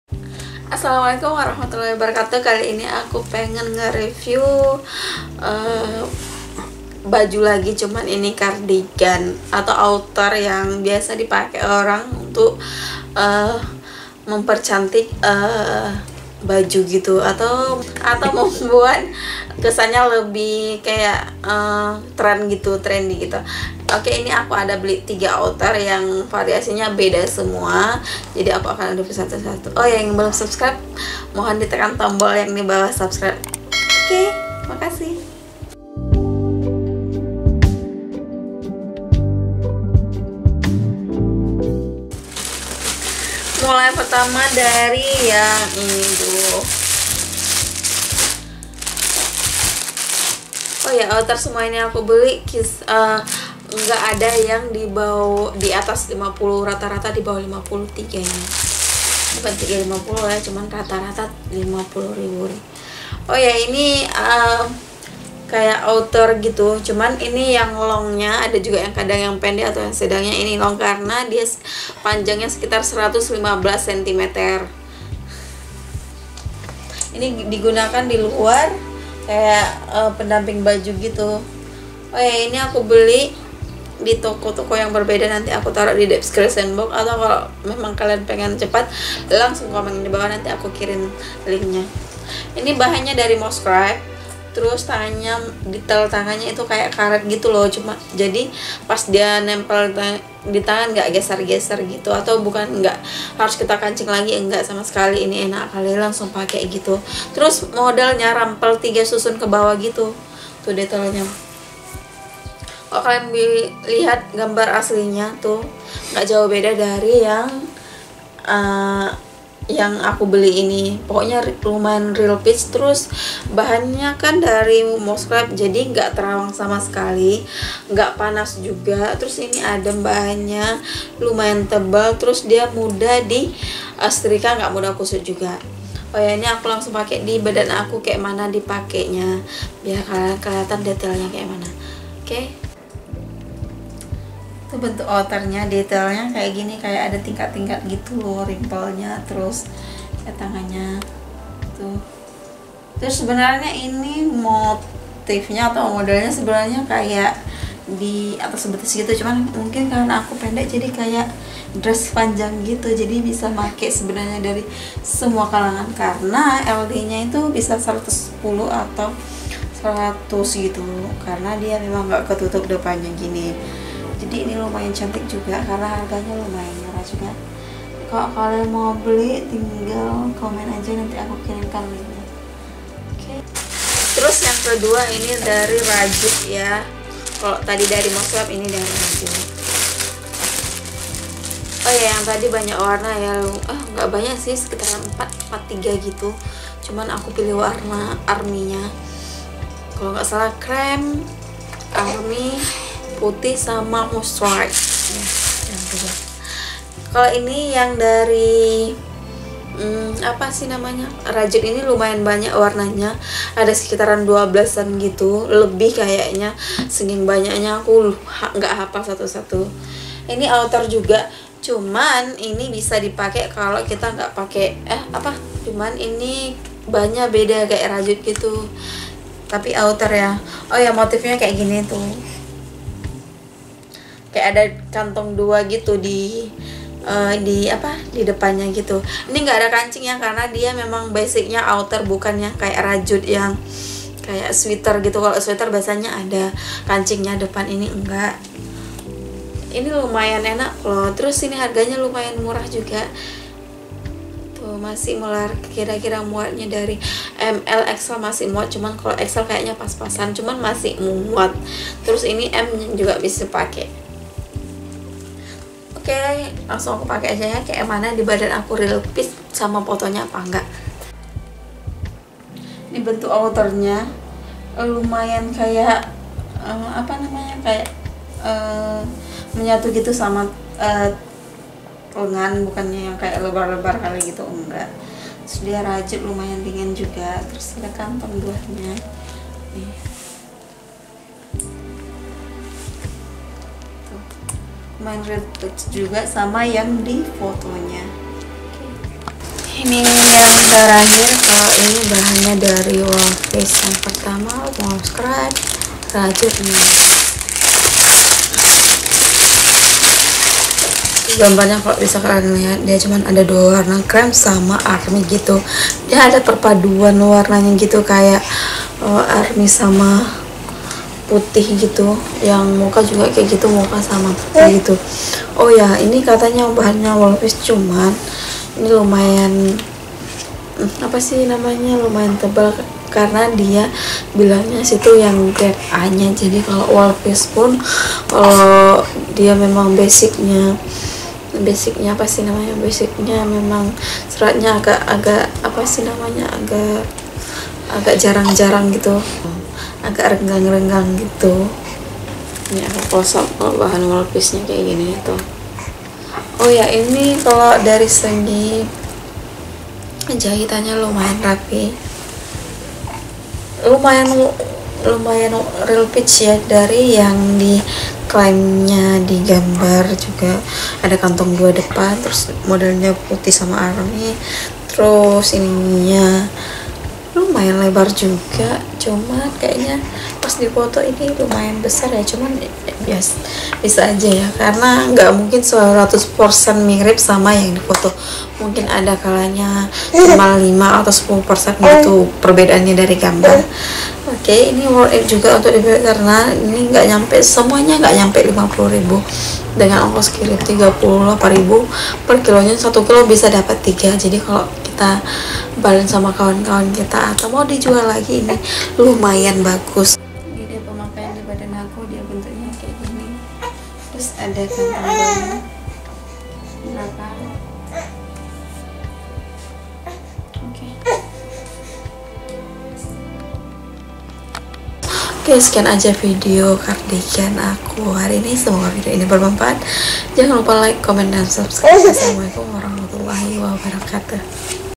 Model Baju Cardigan Terbaru 2020 | Baju Cardigan Rajut | Model Outer Terbaru 2020 | TRY ON
Model Outer terbaru 2020 dengan model Baju Cardigan Rajut dan Cardigan Outer yang fashionable untuk para wanita modis. Simak terus videonya sampai habis ya, agar tidak ketinggalan informasinya.

ladies cardigan baju
Model Baju Cardigan Terbaru 2020
Baju Cardigan Rajut

Produk yang di Review :
1. Kalila Long Outer Rempel Layer

2. Cardigan Wanita Rajut Halus Long Cardigan BELLE OVERSIZE

3. Agni outer / cardigan gradasi / cardigan kombinasi

*Link Toko Online dapat berubah dan tidak aktif
*Harga Produk pada Toko Online dapat berubah tergantung kondisi serta ketersediaan stock barang.

Demikian video review saya...
Jika ada kesalahan kata-kata atau cara bicara yang kurang berkenan mohon dimaafkan, terima kasih